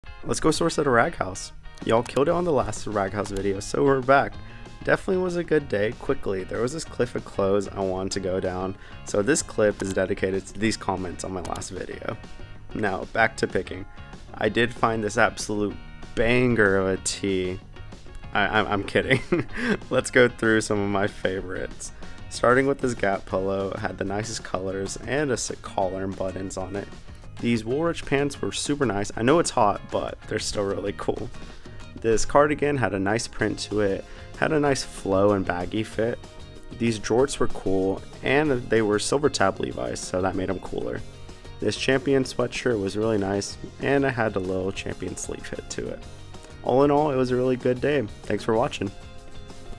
Sourcing at a Rag House looking for vintage! It was hot but found a ton!
Thank you for checking out the Video! To see up to date announcements and web drops be sure to follow me at @resilienthtx on instagram! To shop latest inventory be sure to check out Resilienthtx.com! Thank you for the support (: